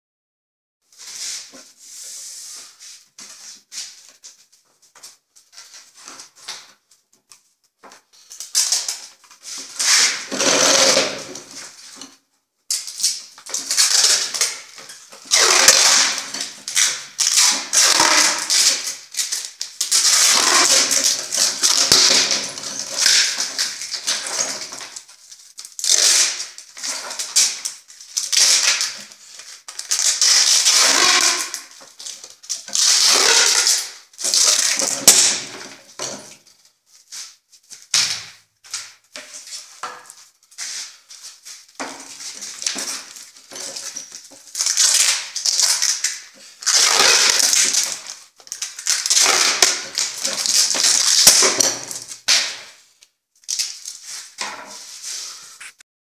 Как связать трубы скотчем своими руками?
Видео показывает, как своими руками связать скотчем трубы, чтобы их было удобно переносить, они не разваливались и не смещались относительно друг друга.